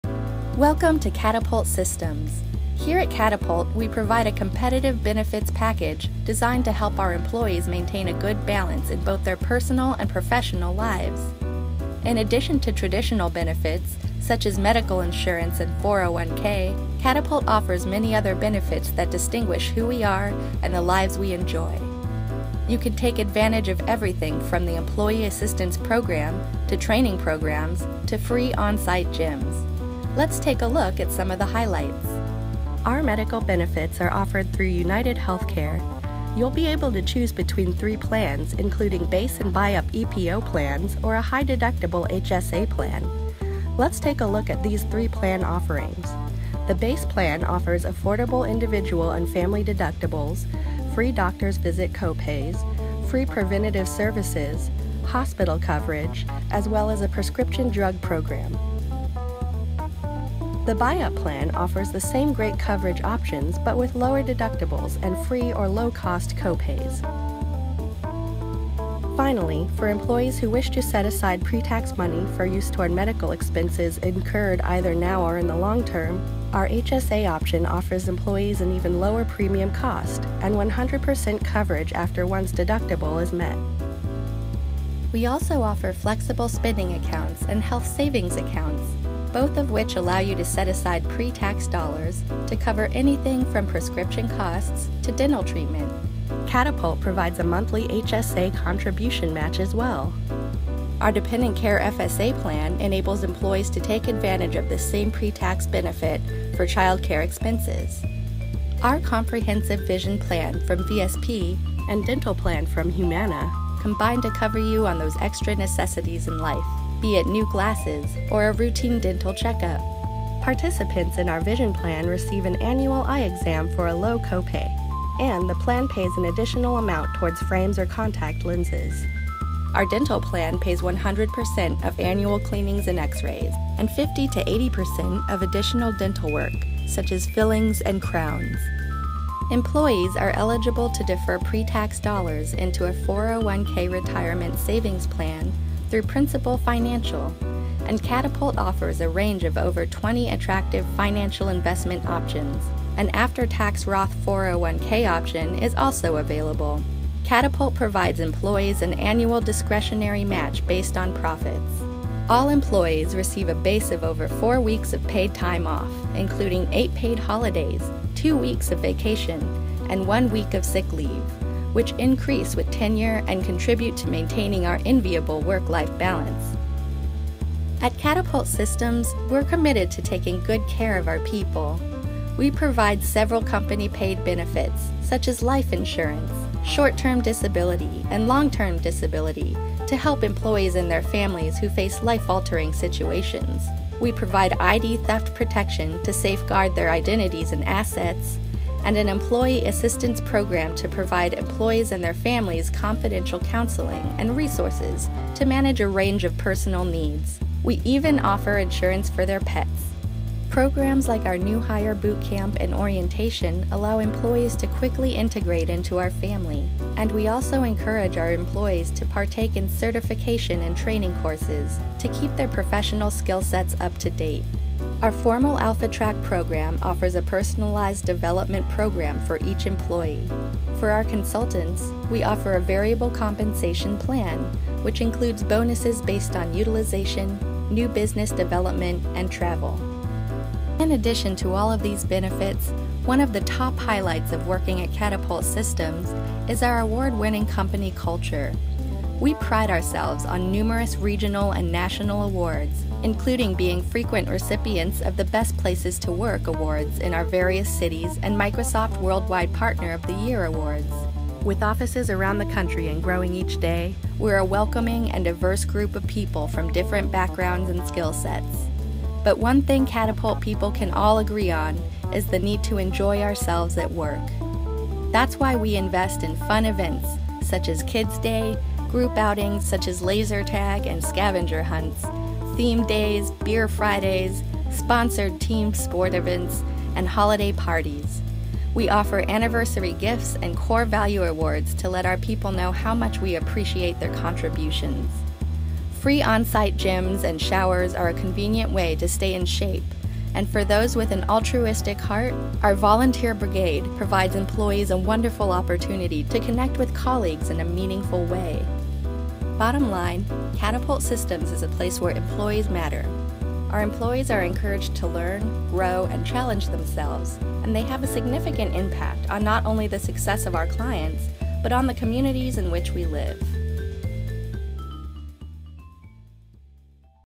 Benefits Overview | Catapult Systems
Catapult provides a competitive and comprehensive benefits package designed to help our employees maintain a good balance in both their personal and professional lives.
In addition to the typical benefits offered, Catapult has many others that distinguish who we are and the lifestyles we enjoy including quarterly fun events, wellness initiatives (with some friendly competition), company-wide bingo on holidays, beer cart Friday and more.